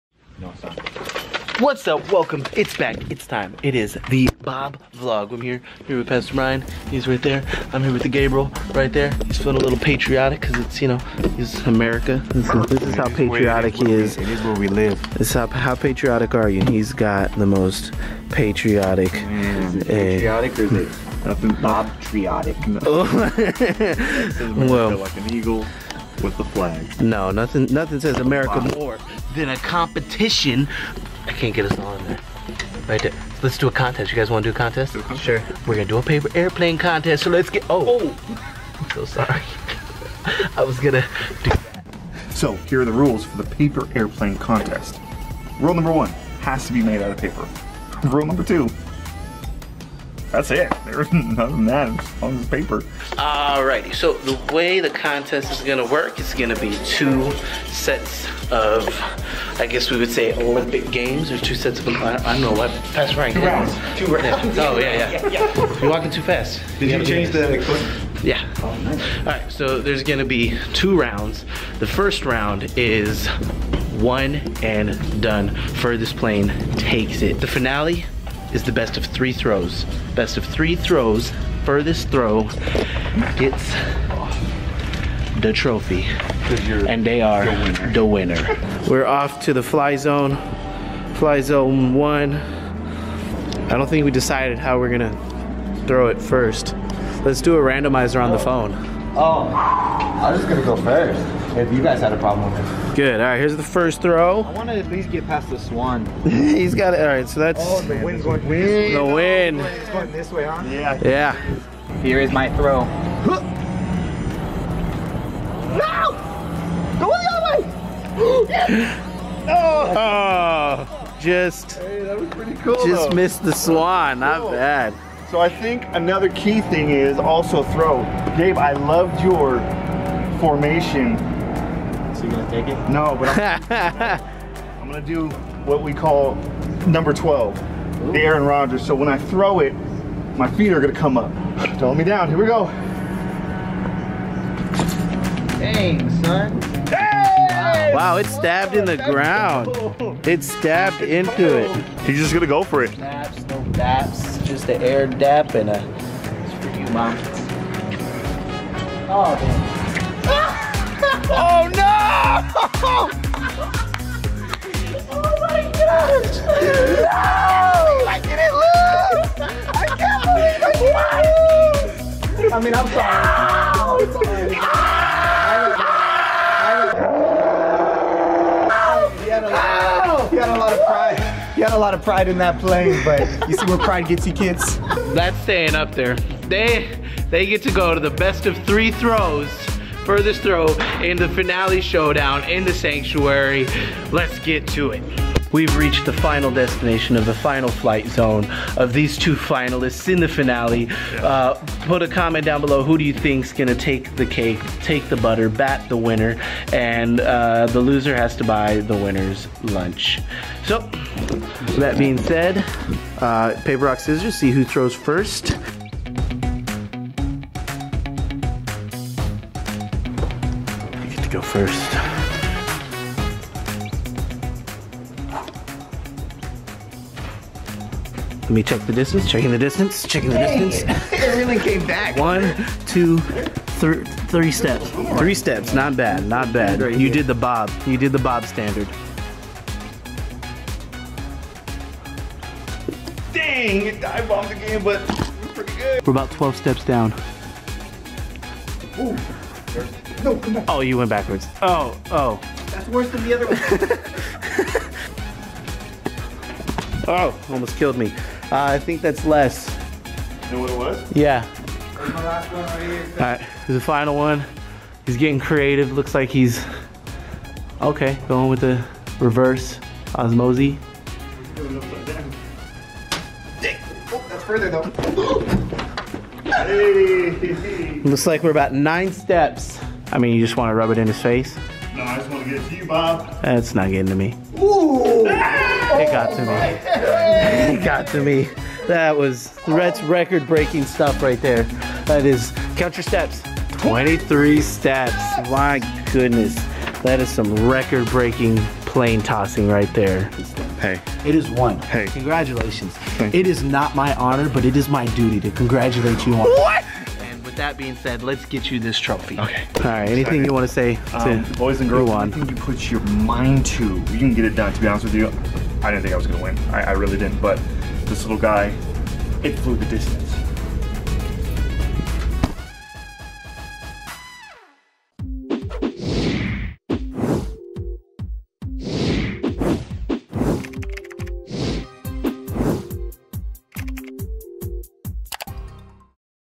Bob Vlog - Paper Airplane Competition!
In this Bob Vlog see Bob's paper airplane skills be put to the test! Who will be victorious? Watch and find out!